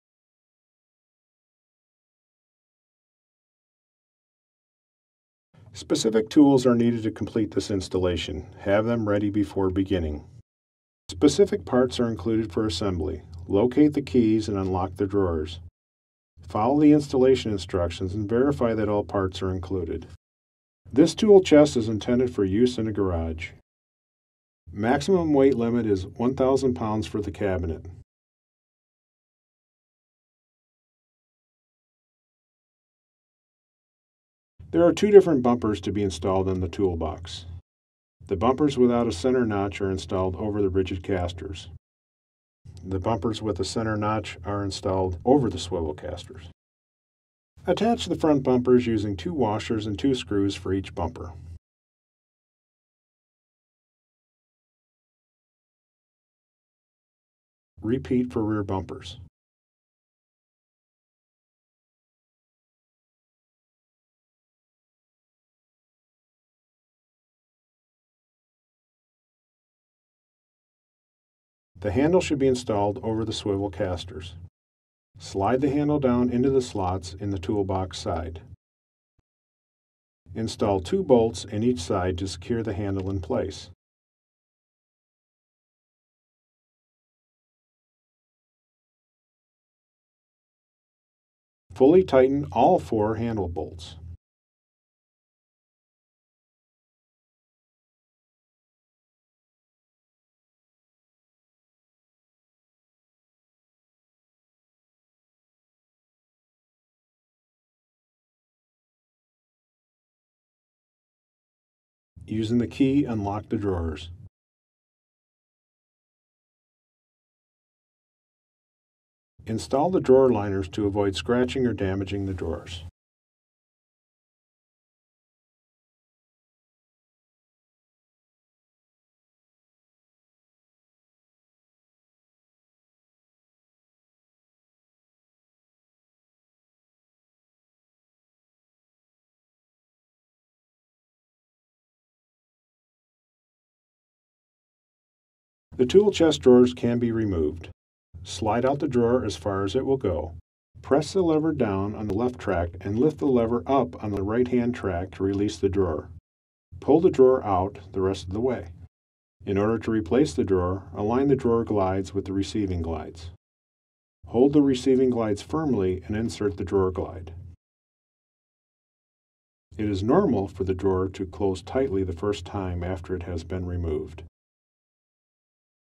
Assembly Instructions for the Gladiator® Premier Series 10-Drawer 52" Roll-Away
Assembly instructions for the Gladiator® Premier Series 10-Drawer 52" Roll-Away tool chest (GATR5210WG).